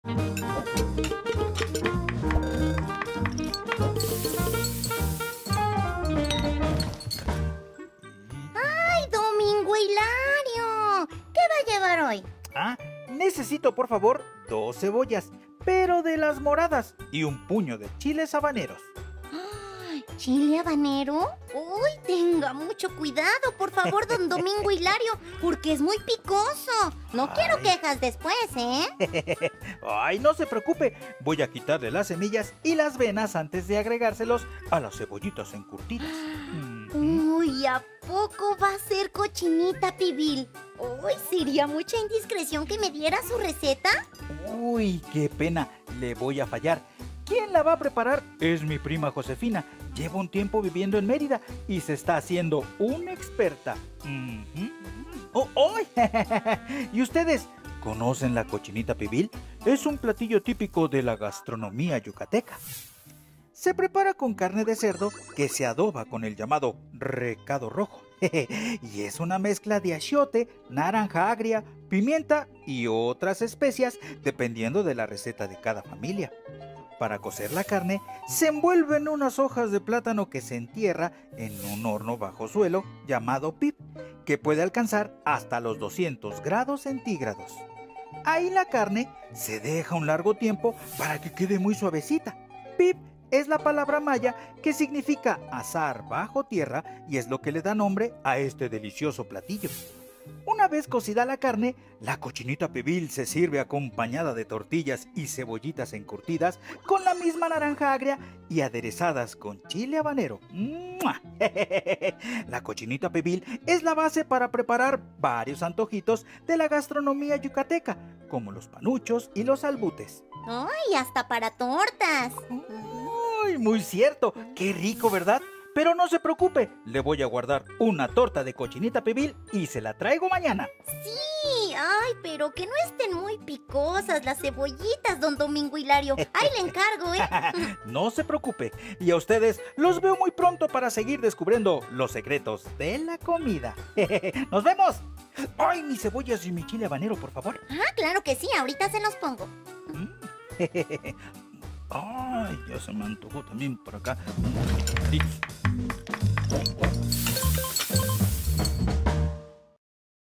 Secretos culinarios de Staff. Cochinita Pibil
La cochinita pibil es un platillo emblemático de la gastronomía yucateca, conoce su origen.